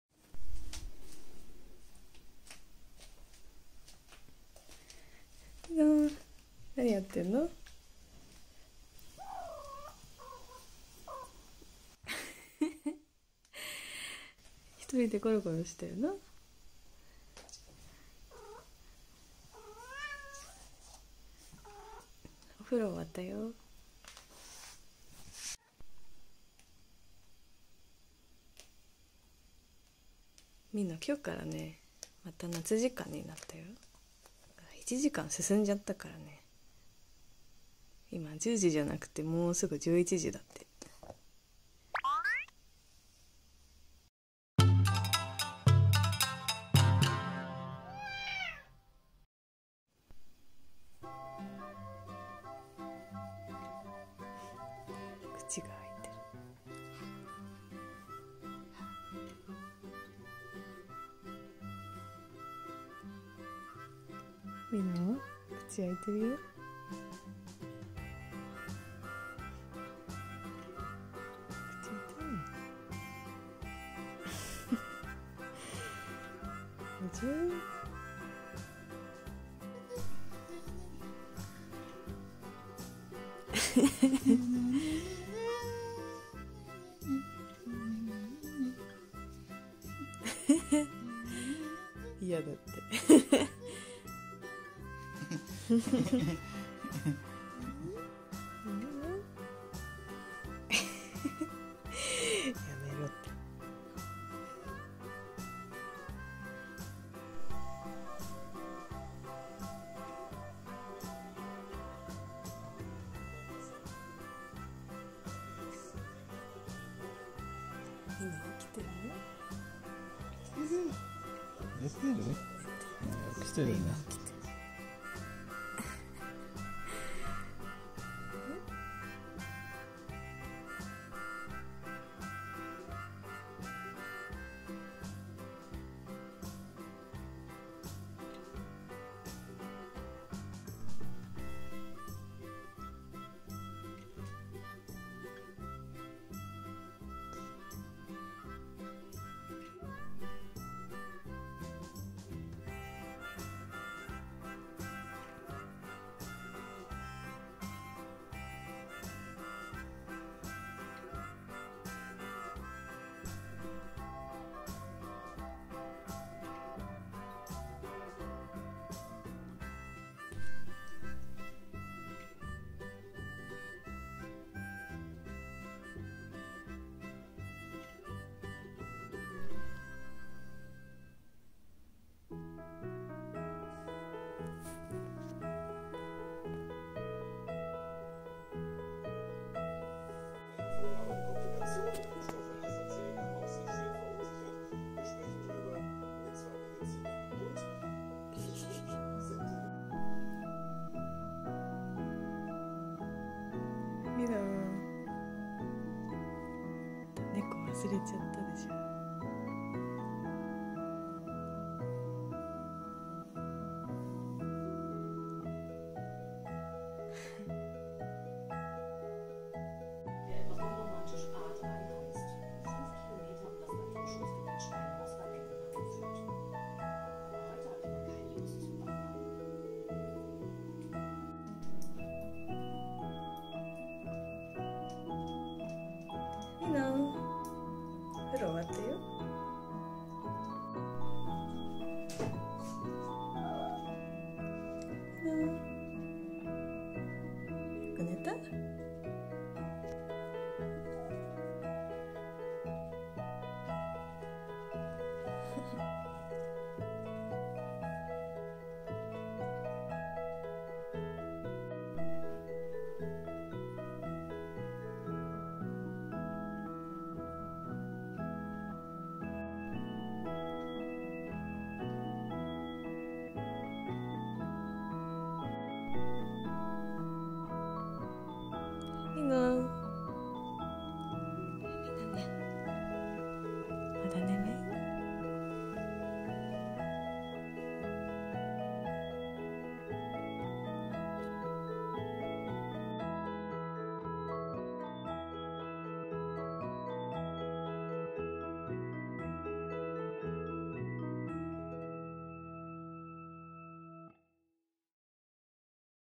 Cat Doing NOTHING for the Whole Day - Mino is Sleepy at Home
In this video you will see how our cat Mino spends a regular day in his life on a Caturday doing nothing from morning till evening while being very sleepy for the whole day. Mino is a extraordinary talkative siamese mix breed house cat who lives with us in Germany. He has many unusual and surprising behaviour patterns, quite different to other cats, that you will experience in this video. This video is in Japanese with English subtitles.

I hope you can relax and enjoy this video.